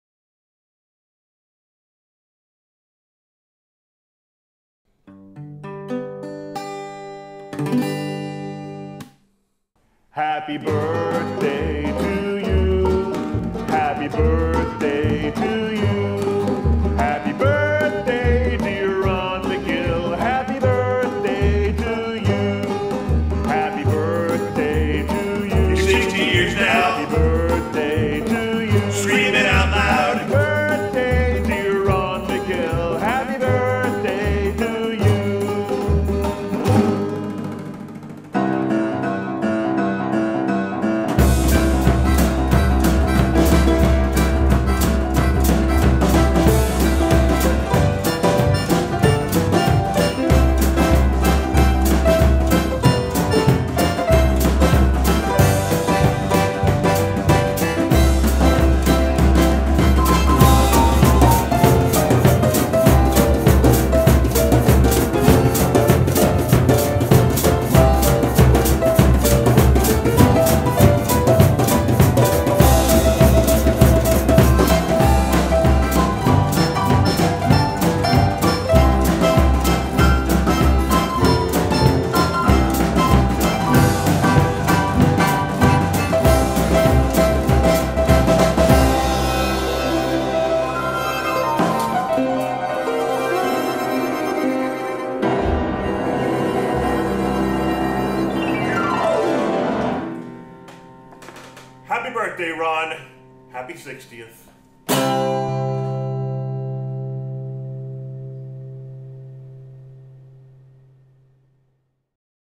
HAPPY BIRTHDAY RON MAGILL
A 60TH BIRTHDAY VIDEO TO MY FRIEND RO MAGILL